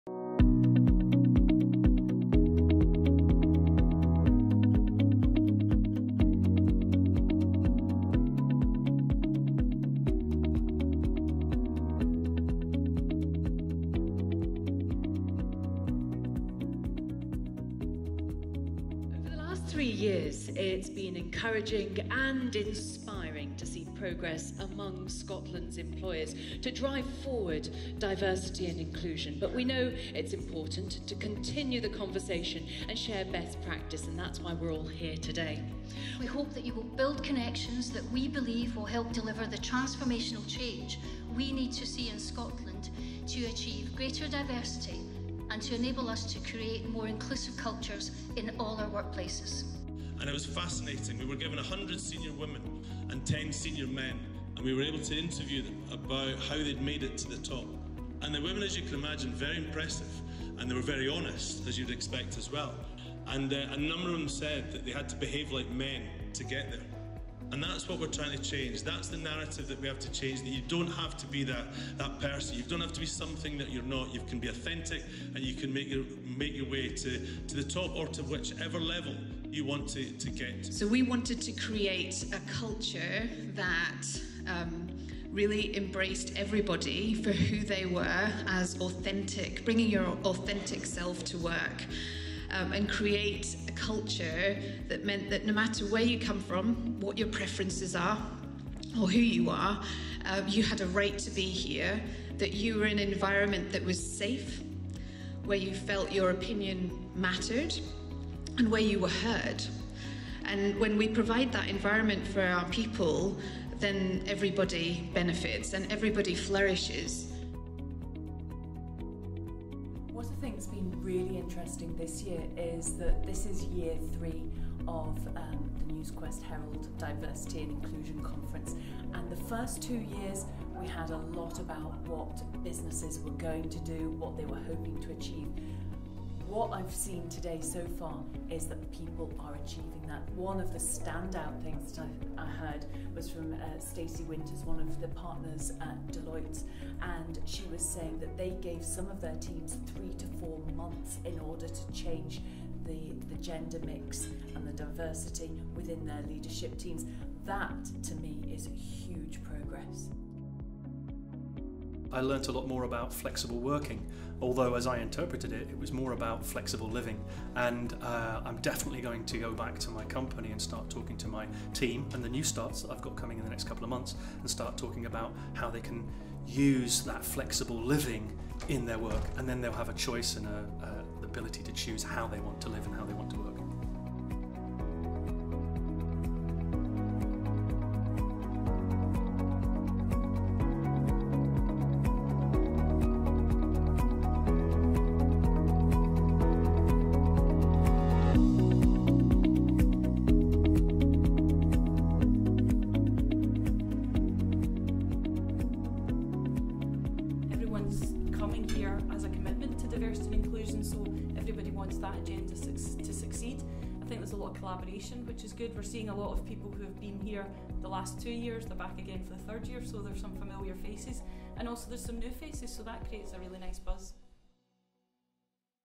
The Herald and GenAnalytics Diversity Conference 2019
The Herald and GenAnalytics held Scotland’s third national Diversity Conference to help achieve Scotland’s ambitions for inclusive economic growth on Tuesday, May 7, 2019. The conference was a platform for organisations to learn more about how to embrace cultural change and build the capability within their management and leadership teams to become more inclusive, not just in the workplace, but within our societies and the country as a whole. The conference attracted business and public sector staff, third sector and charities, with responsibility for equality, diversity and inclusion strategy and implementation.